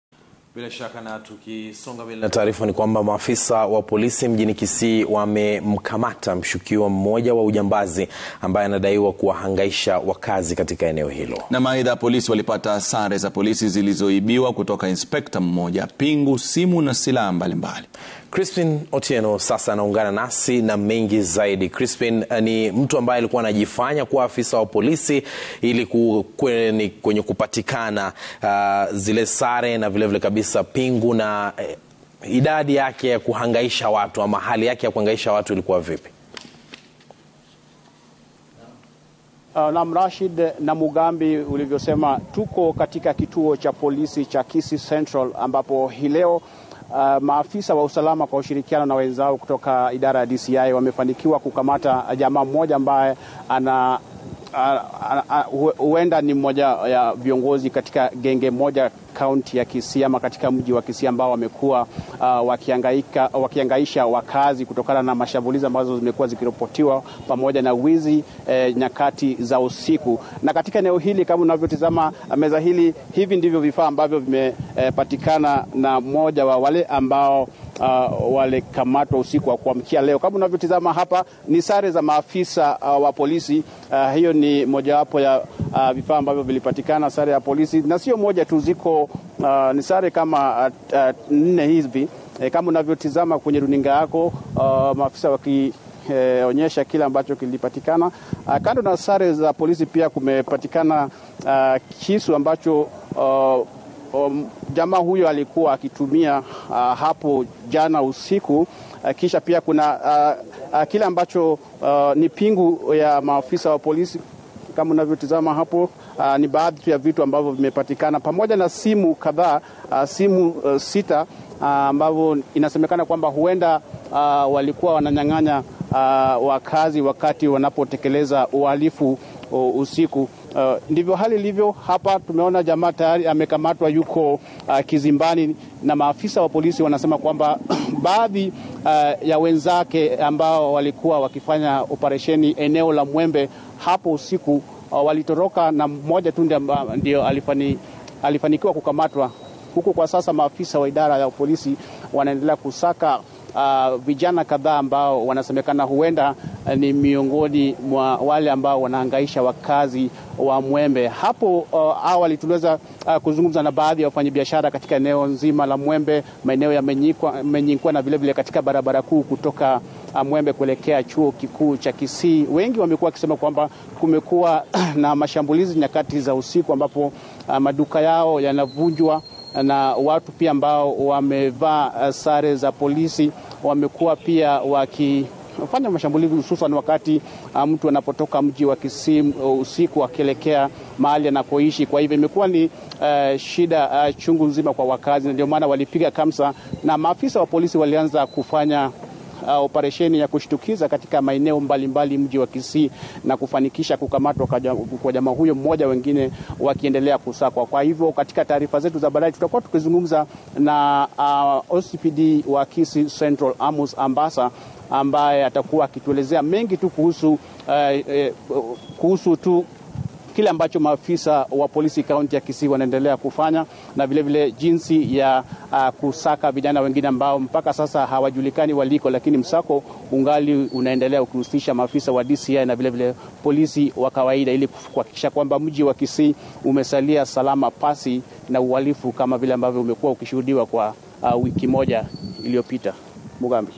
Maafisa wa polisi mjini Kisii wamkamata mshukiwa wa ujambazi
Maafisa wa polisi mjini Kisii wamemkamata mshukiwa mmoja wa ujambazi ambaye anadaiwa kuwahangaisha wakazi. Aidha polisi walipata sare za polisi zilizoibiwa kutoka inspekta mmoja, pingu, simu na silaha mbalimbali